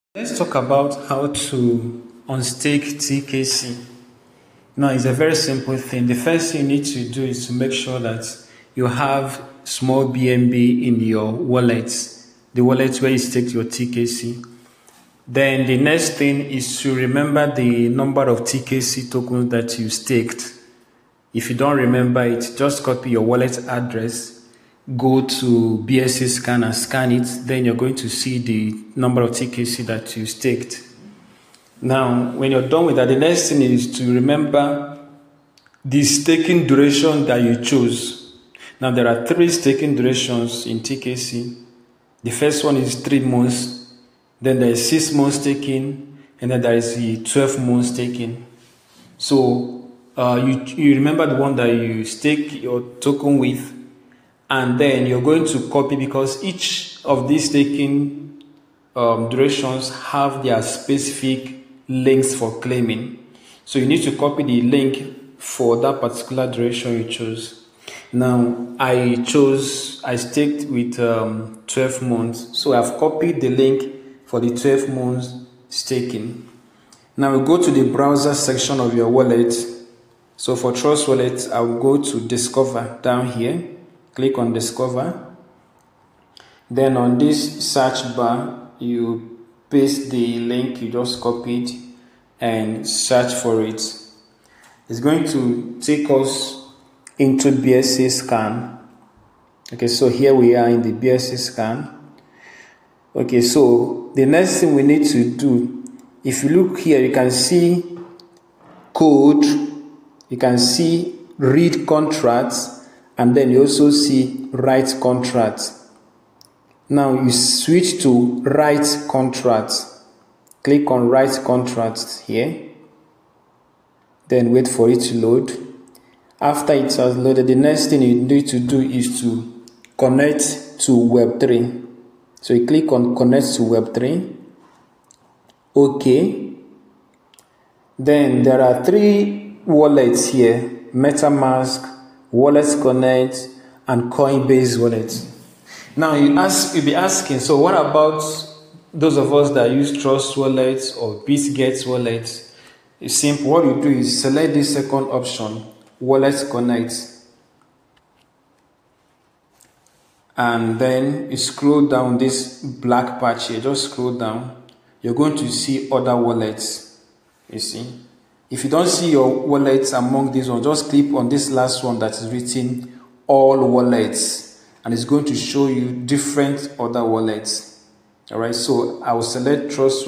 How To Unstake TKC On Trustwallet
Learn how to unstake TKC on Trust Wallet with our step-by-step guide. Follow our tutorial to safely and easily unstake your TKC tokens and manage your cryptocurrency portfolio. Watch now and take control of your TKC assets.